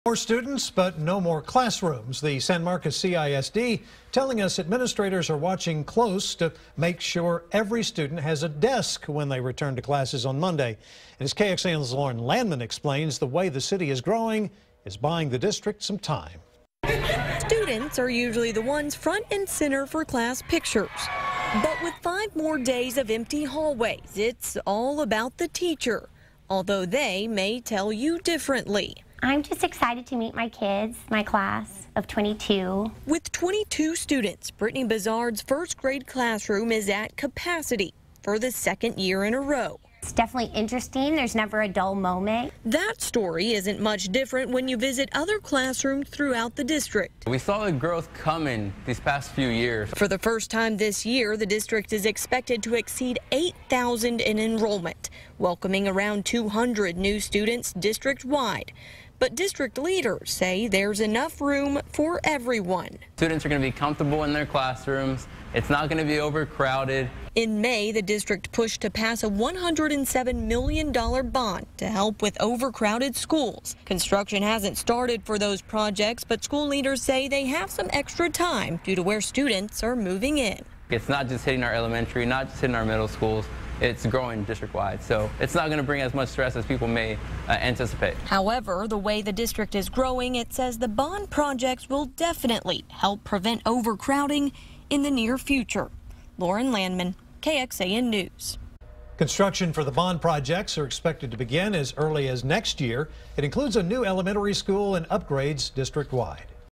San Marcos CISD prepares for new school year, more students
The district reports it had around 7,700 students enrolled for the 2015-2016 school year, this year it is prepared to exceed 8,000.